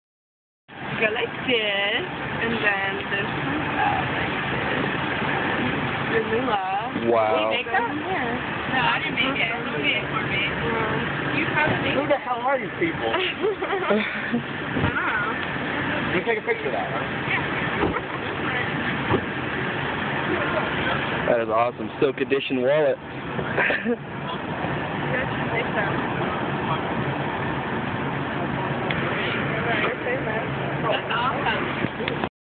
Amazing DIY Wallet made from an empty silk milk container.
I was working on a promotion for silk milk and two these girls drove up and just happened to have a wallet made from an old silk milk container. What are the odds?